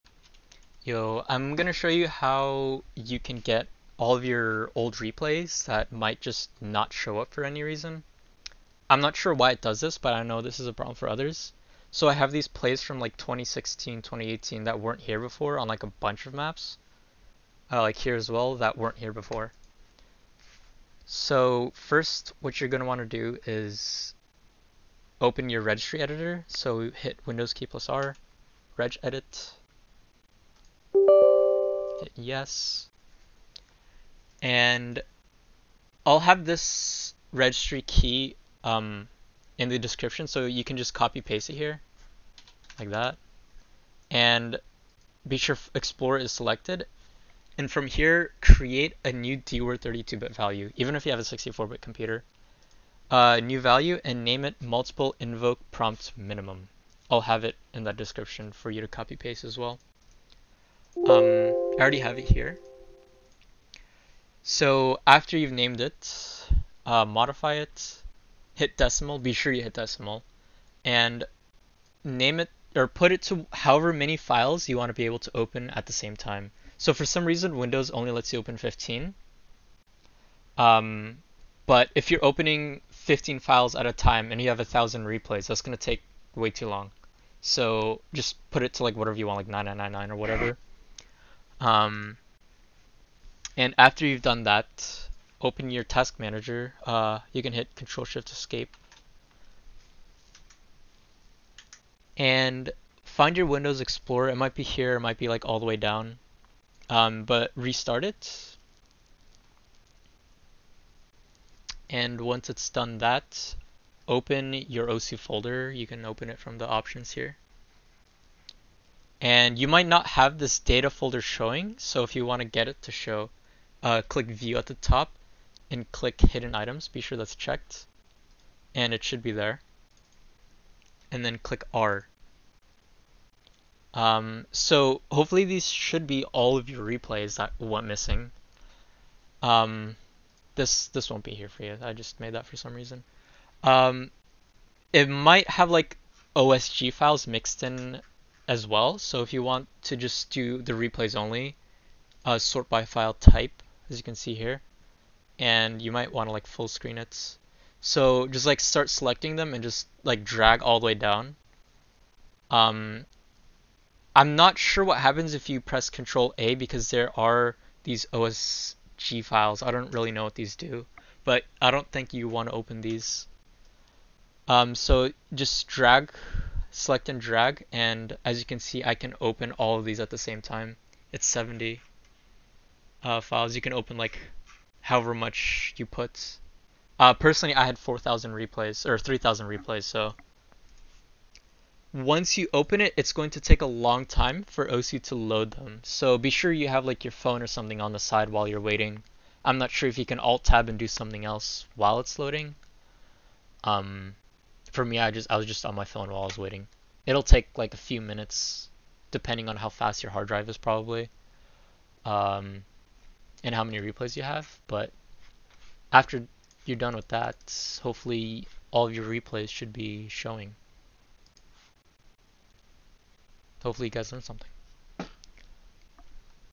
How to find your missing osu! replays
Computer\HKEY_CURRENT_USER\SOFTWARE\Microsoft\Windows\CurrentVersion\Explorer

MultipleInvokePromptMinimum